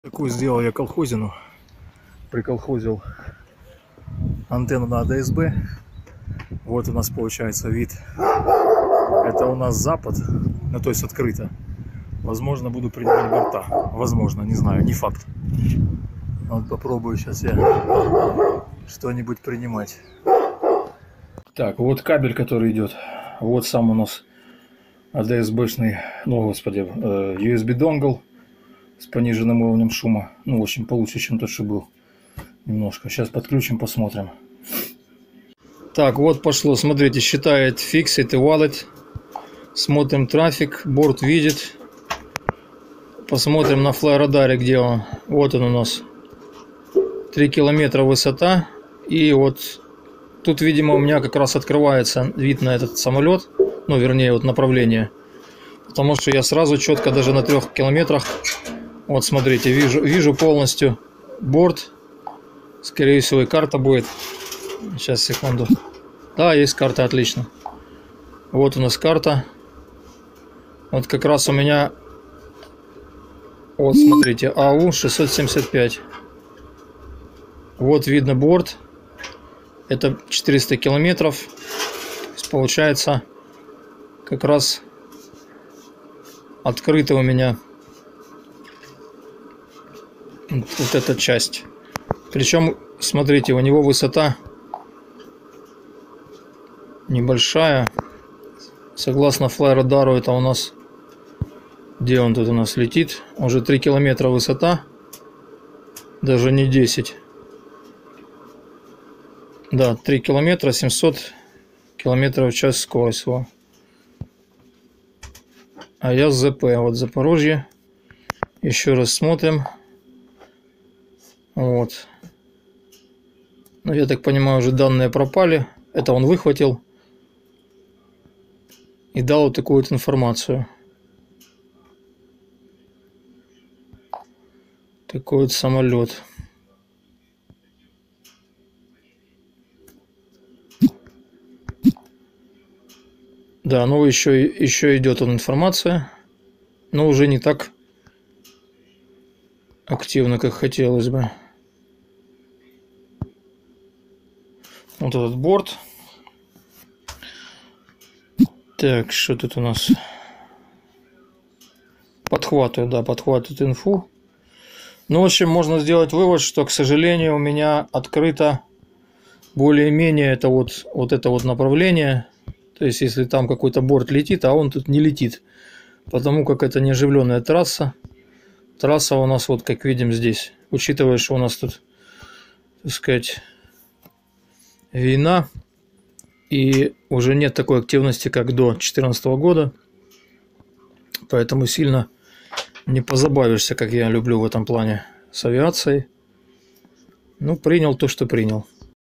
Супер тест авиа антенны для 1090 МГц ADSB
Вот ролик где показана сама антенна и в описании под роликом указана ссылка на конструкцию антенны А вот пример на сколько далеко эта антенна может принимать борта .Самолёты видно уже над Ростовом